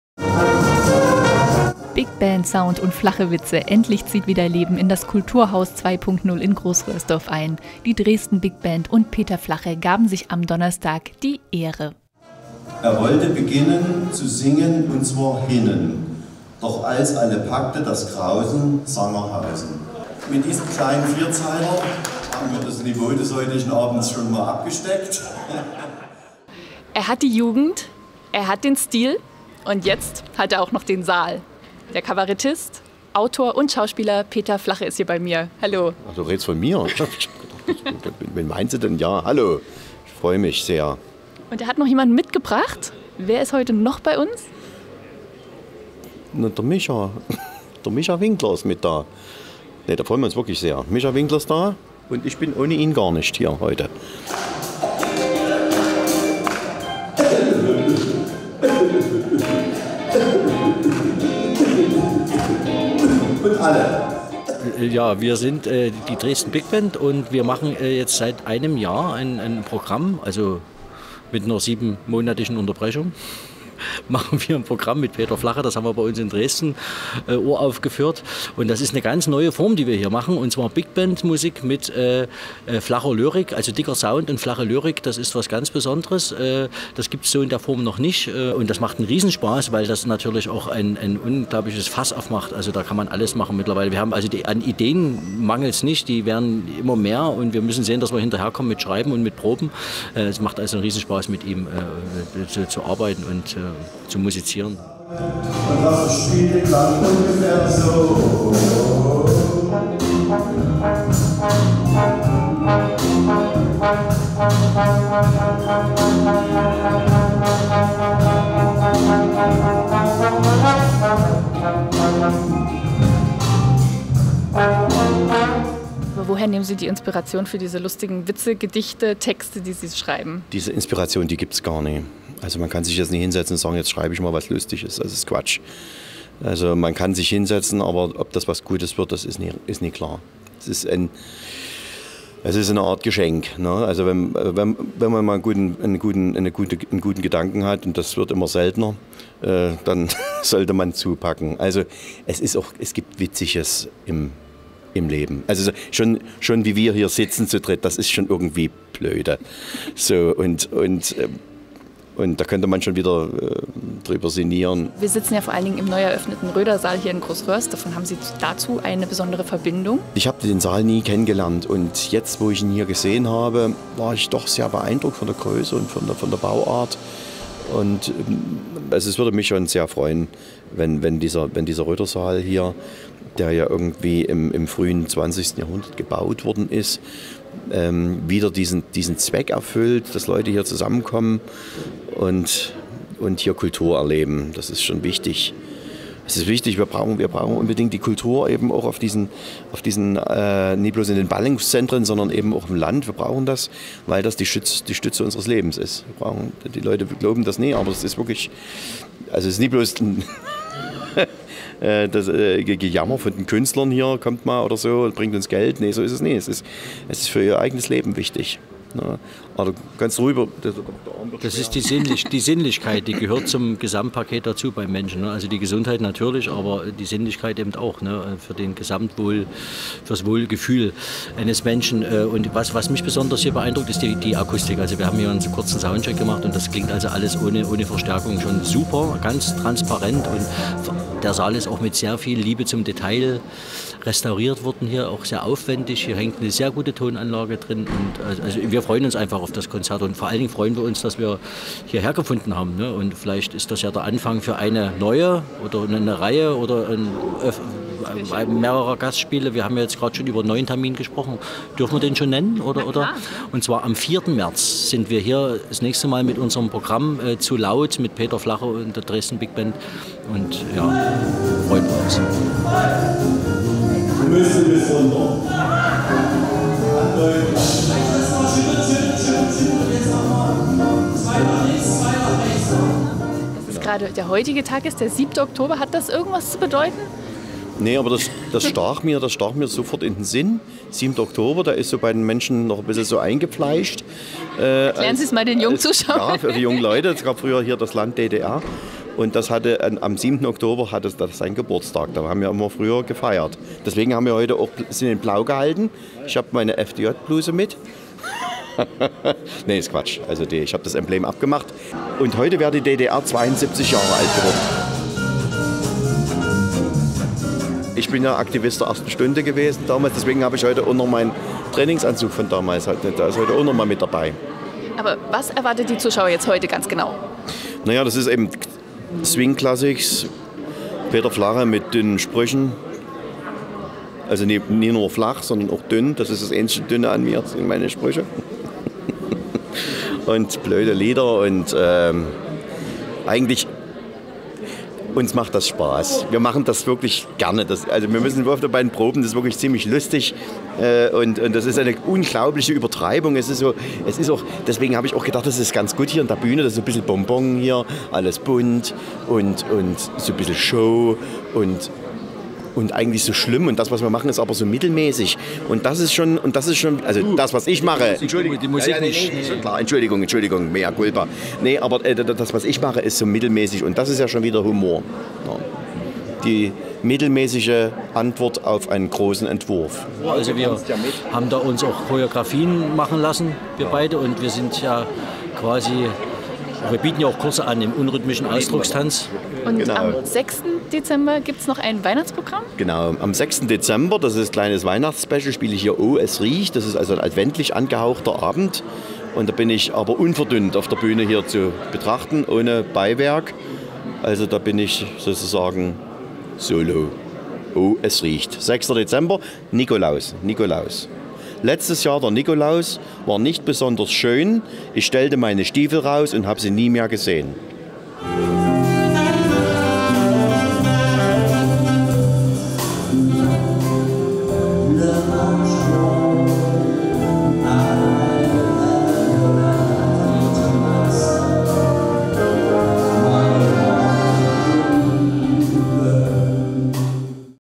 Großröhrsdorf: Peter Flache im RoederSaal - LAUSITZWELLE
Der kürzlich neu eröffnete RöderSaal in Großröhrsdorf hatte vergangenen Donnerstag wieder einen wunderbaren Gast im Haus. Der beliebte sächsische Kabarettist Peter Flache begeisterte sein Publikum gemeinsam mit der „Dresden Bigband“ im neu hergerichteten Jugendstilsaal. Wir waren vor Ort und haben mit den beiden Künstlern gesprochen.(fk)